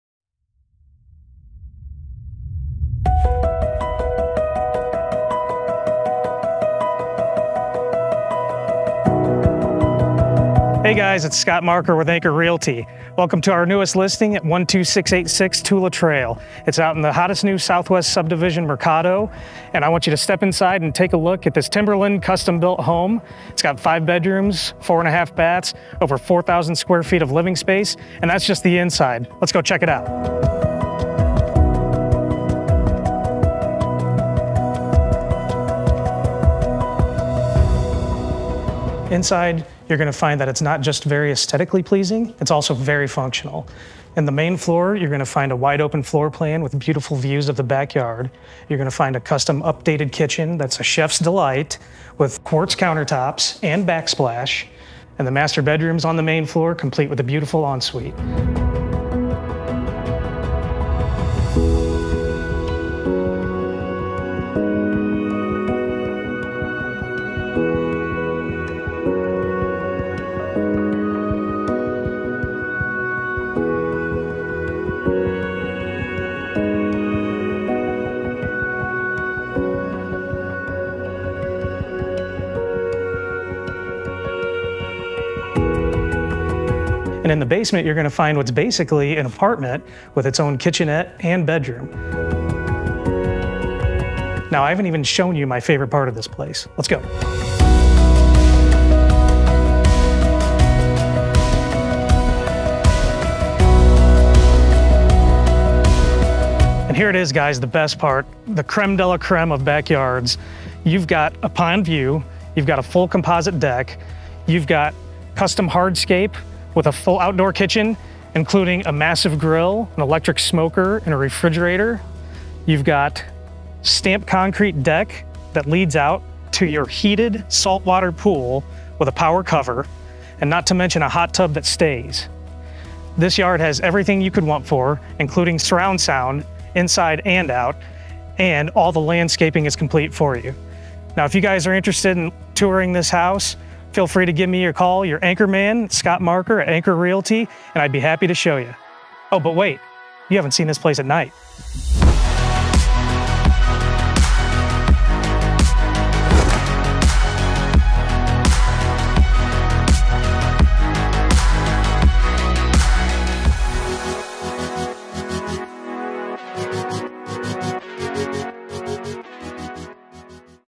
Luxury Living in Southwest Allen County - 12686 Tula Trail
Welcome to your opportunity to own an unparalleled residence in Fort Wayne's prestigious Mercato development. This custom Timberlin home boasts 5 beds, 4.5 baths, and over 4000 sqft of luxurious living space. A model of sophistication, this home offers an exquisite interior paired with an outdoor paradise. Approaching this residence situated on a corner lot, you're greeted by tasteful landscaping and an aesthetically pleasing front elevation featuring Board and Batten siding hinting at the elegance within. Step inside to discover a spacious open floorplan bathed in natural light, accentuating the elegant light oak flooring throughout the main level. The great room impresses with soaring beamed ceilings, a charming brick fireplace, and built-in shelving. Entertain with ease in the kitchen and dining area, complete with a large island, custom cabinets, quartz counters and matching full-height backsplash. Added luxuries include an RO system for purified water and a whole-house softener. Conveniently situated on the main floor, the master suite offers a serene retreat with beamed, tray ceilings, an en suite bath featuring dual vanities, a tile shower, and a walk-in closet flooded with natural light. Upstairs you'll find meticulously crafted details such as a shiplap ceiling in the landing and modern lighting. Two bedrooms share a Jack and Jill bath, while the third enjoys its own private en suite. The expansive basement presents a versatile living space, complete with a bedroom, full bath, and kitchenette, ideal for hosting guests or accommodating multigenerational living. Step outside to discover the true gem of this home – an outdoor sanctuary designed for ultimate relaxation and entertainment. A composite deck leads to a fully fenced yard with stunning pond views. The custom hardscaped patio features an outdoor kitchen with a massive grill, refrigerator, and smoker. Descend to the stamped concrete deck surrounding the in-ground, heated saltwater pool, complete with fountains and lights for added ambiance. The included hot tub ensures year-round luxury. Enjoy Bluetooth surround sound both indoors and out. Other valuable features include a full home generator, HEPA-UV air filtration system, new sod and irrigation system, 3 car garage, mud room with built in lockers and much more. Conveniently located within walking distance of Southwest Allen County Schools, trails, and amenities, this home offers an unmatched lifestyle that is both lavish and rare to find!